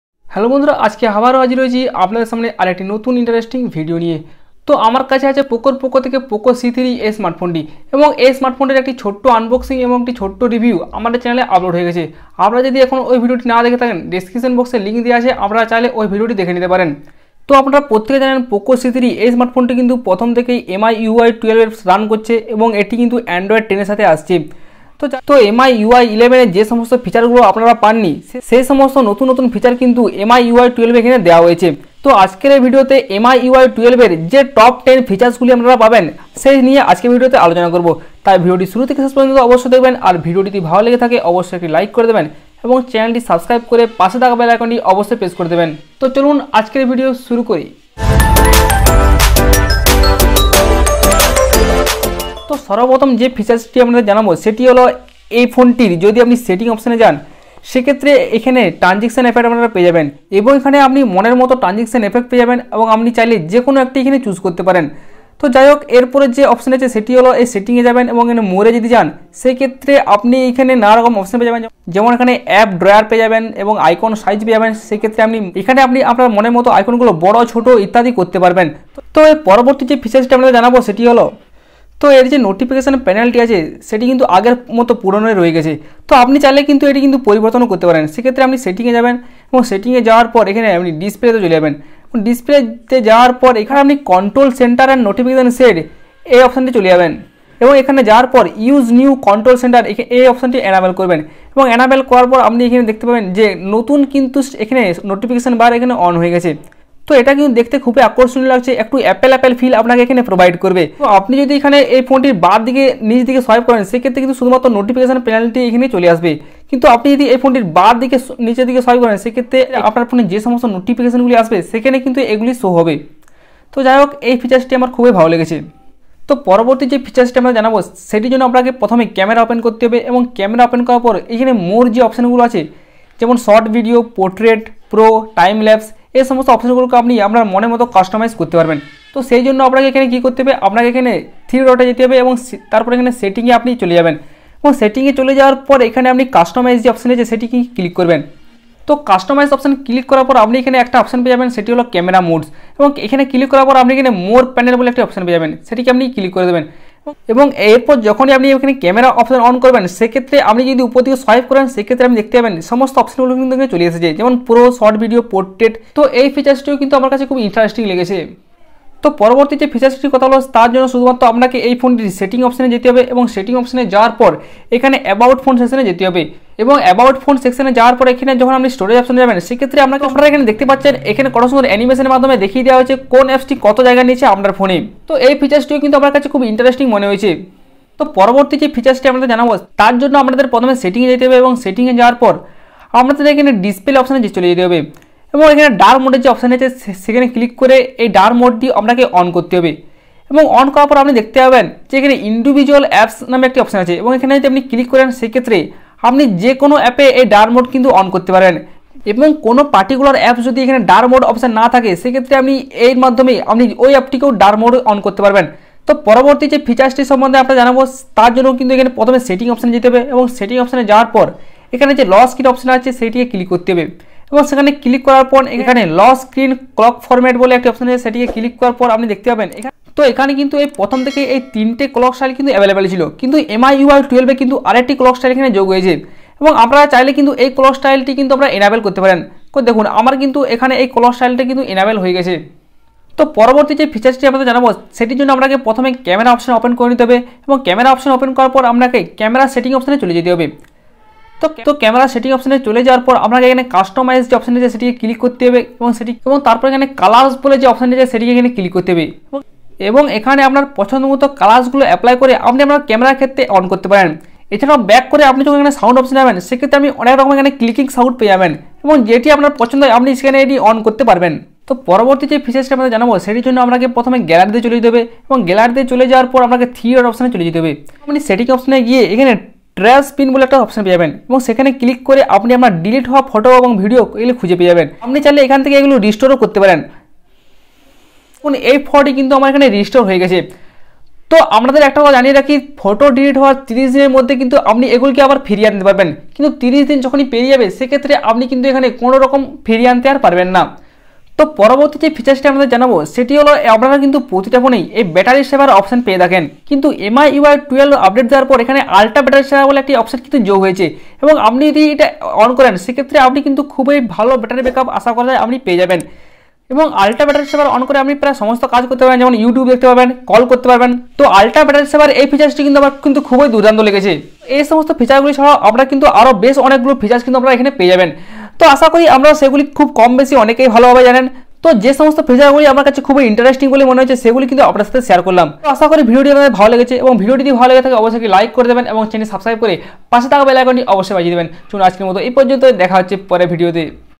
Top 10 new MIUI 12 hidden features in poco C3 | MIUI 12 Features in bangla...
Poco C3 unboxing & review👇👇
In this video we talk about Top 10 new MIUI 12 features.
A number of changes have been made to MIUI 12, such as the notification bar.  Many better features from MIUI 11 have been used here.i really enjoy MIUI 12.